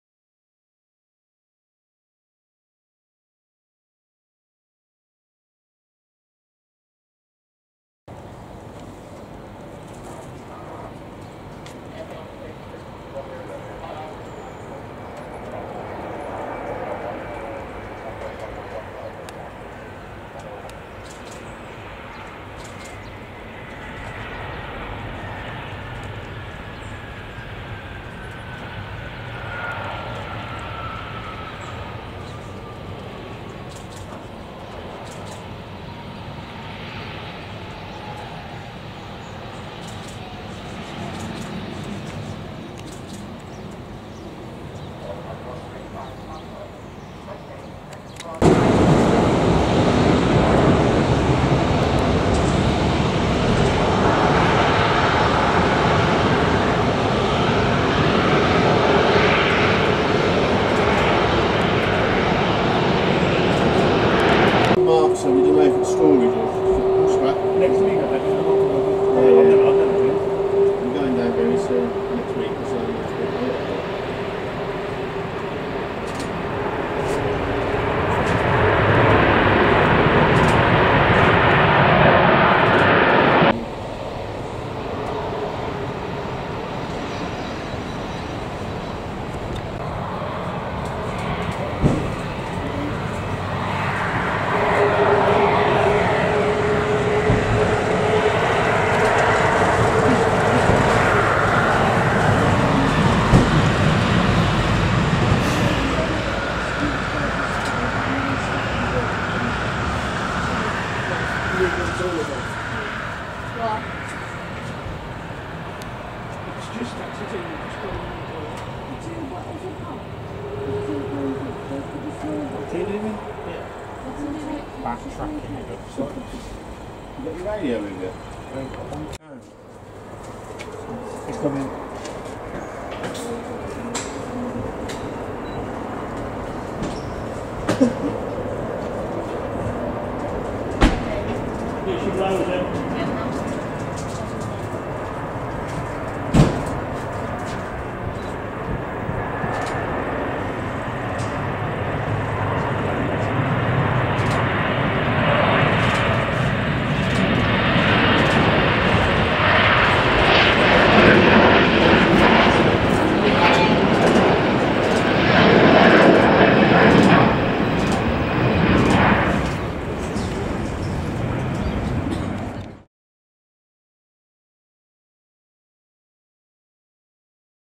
**Rare** Air Mediterranean 737-400 arrival and departure from Birmingham on 15/3/2020
On the same day as A340. This Air Mediterranean 734 dropped in. It came in at 8:30am from Create and departed at 11:42am to Athens. This 734’s are rare sight now. The aircraft was 22 years old. Very nice to see something different and Air Mediterranean only has a fleet of 3 of these and operate charters with them so very nice sight indeed especially when it came in unexpectedly.